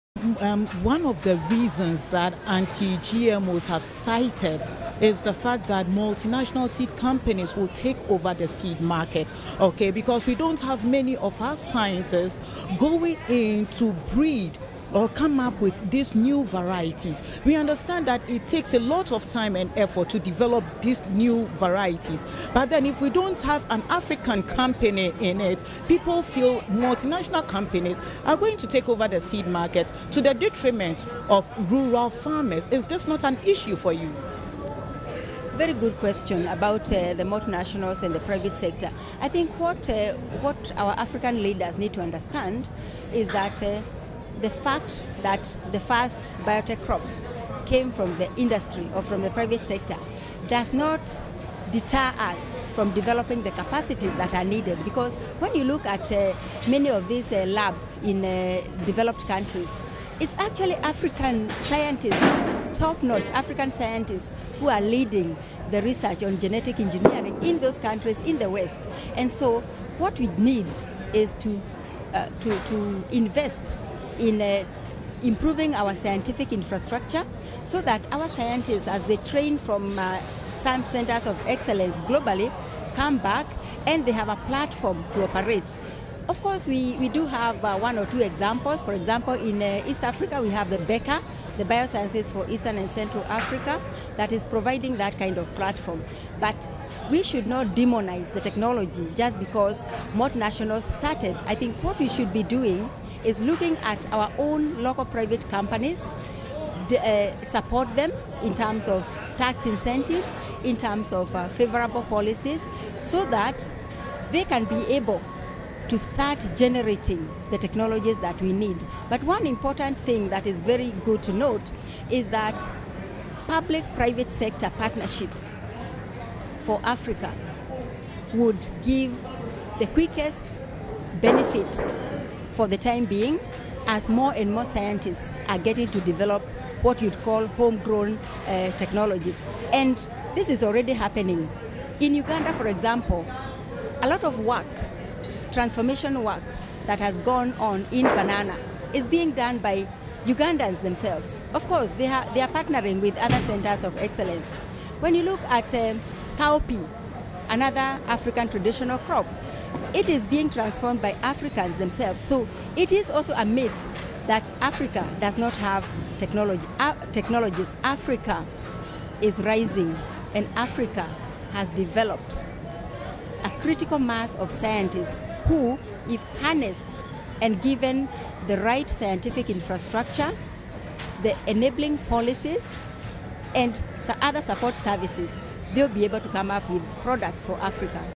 Do multinational companies impede GM crop progress in Africa?
In Part 2 of our interview with Dr Margaret Karembu of the ISAAA AfriCenter, she addresses the question of whether multinational companies impede African progress vis-a-vis GM crops.  She finds that this polarised view does not have to be the case.  African scientists and companies can successfully collaborate with larger companies and are in fact leading the research into African crops.  Moreover, African public bodies' support for research and workable policies are crucial to GM crops in Africa.

Interview conducted by Adelaide Arthur, Joy News (Ghana) at the recent 2014 AAAS conference in Chicago.